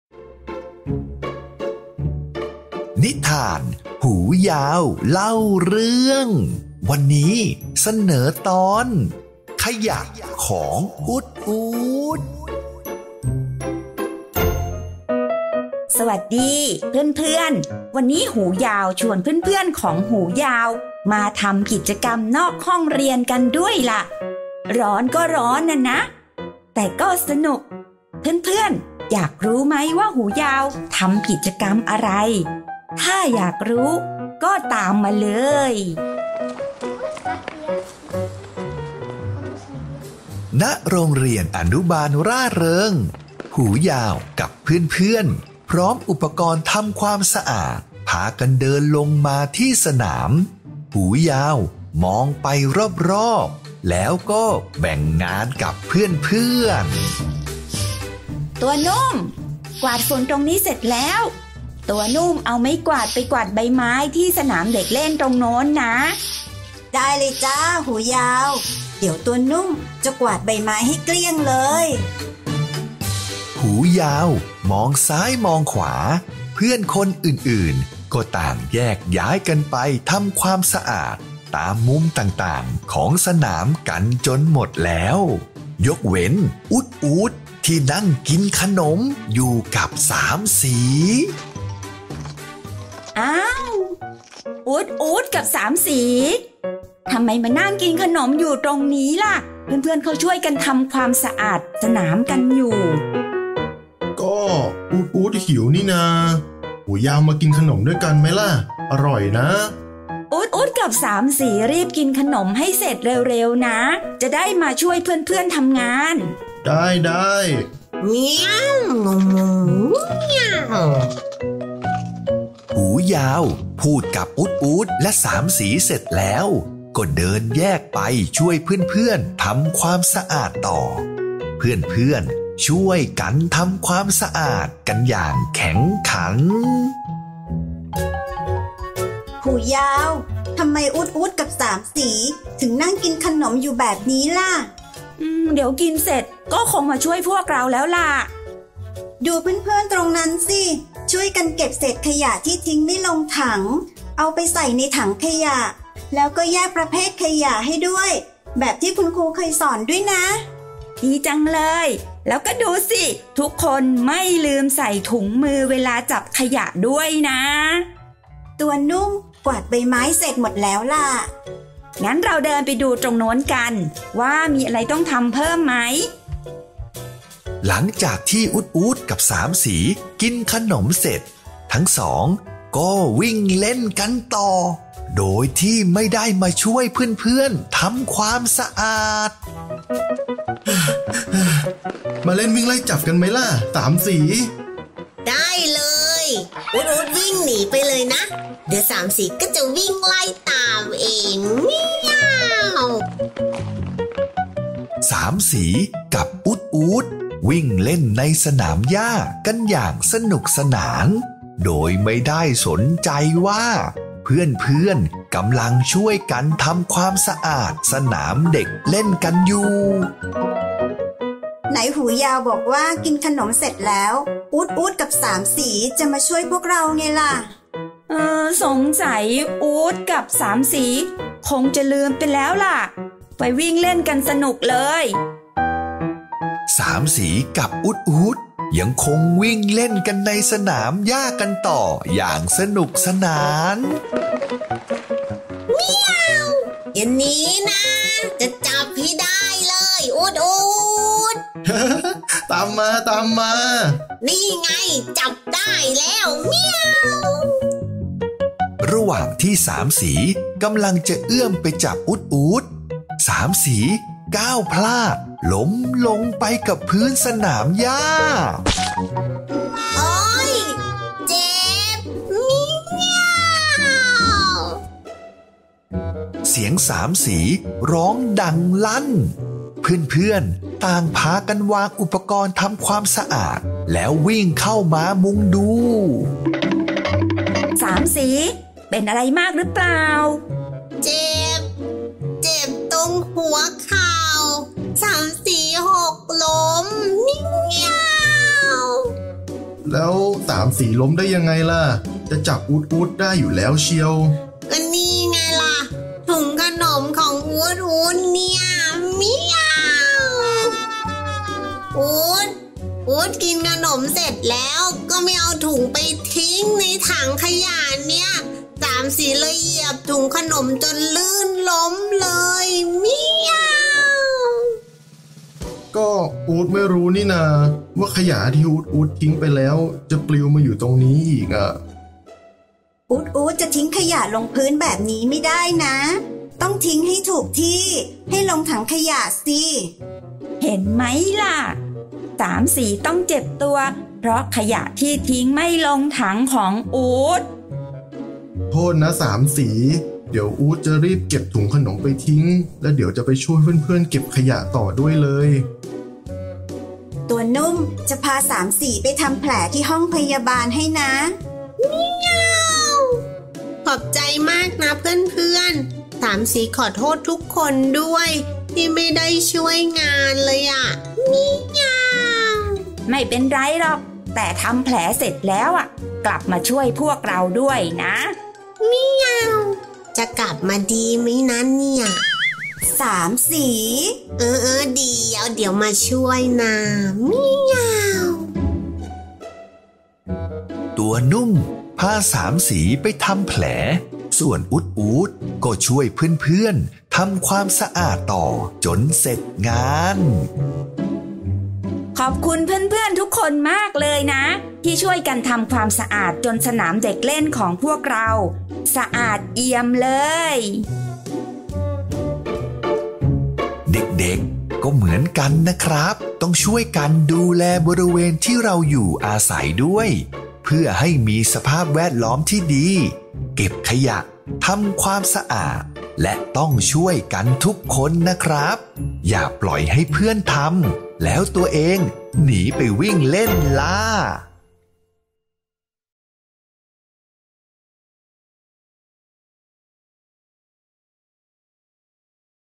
นิทาน ขยะของอู๊ดอู๊ด | หูยาวเล่าเรื่อง
วันนี้หูยาวชวนเพื่อน ๆ มาช่วยกันบำเพ็ญประโยชน์ ด้วยการทำความสะอาดสนามเด็กเล่น และสนามหญ้าที่โรงเรียน แต่สามสีกับอู๊ดอู๊ดไม่ยอมมาช่วยเพราะมัวแต่วิ่งเล่นกันอยู่ แต่แล้วสามสีดันลื่นล้มเพราะเหยียบถุงขนมที่อู๊ดอู๊ดทิ้งเอาไว้ซะได้
หูยาวเล่าเรื่อง ตอนนี้จะพาทุกคนมาช่วยกันรักษาความสะอาด และรู้จักทิ้งขยะให้เป็นที่กัน